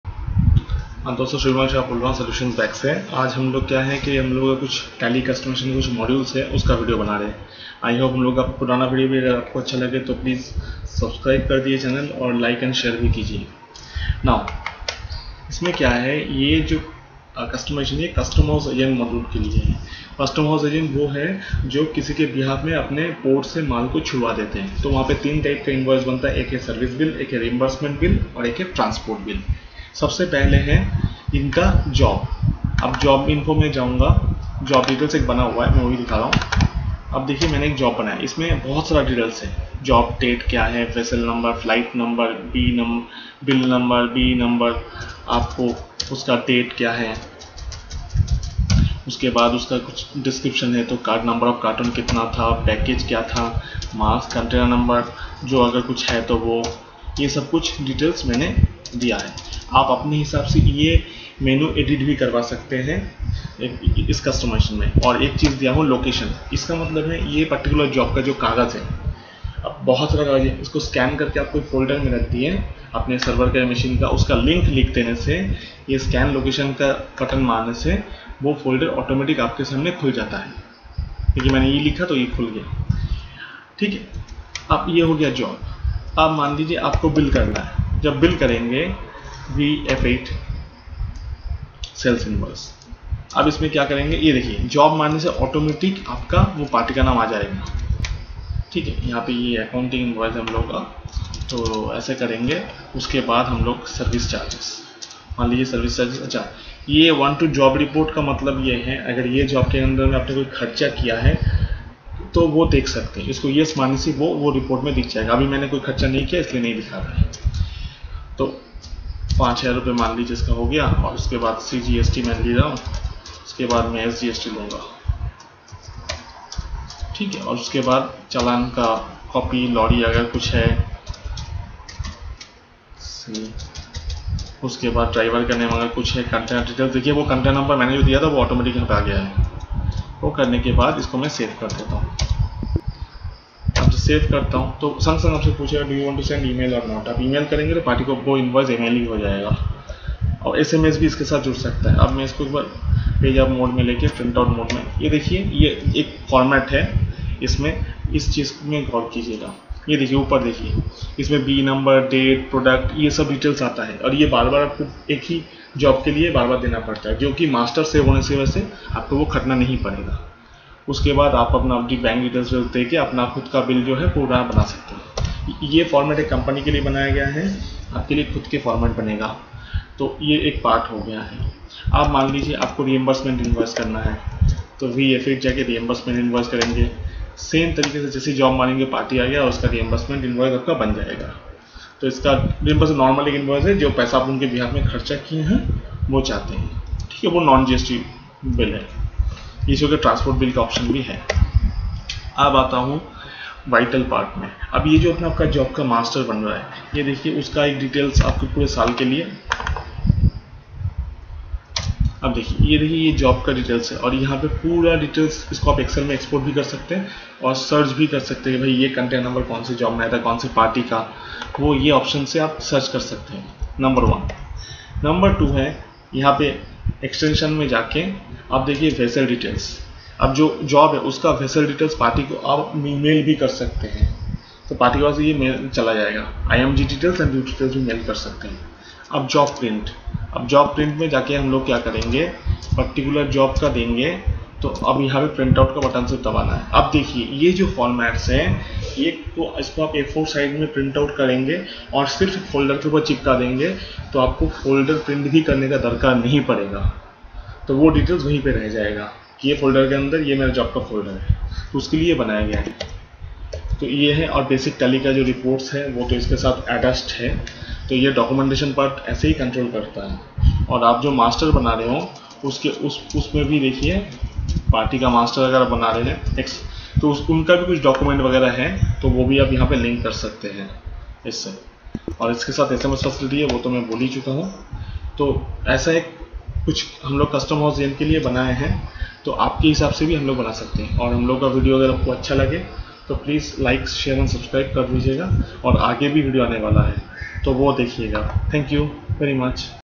Tally Module - Custom House Module by Tally Customization. Price as per Requirement.
Here we will show you,  how a Custom house agent maintain job and billing system in Tally ERP 9.

Bank Details
SOLUTIONS BAG
ICIC0001286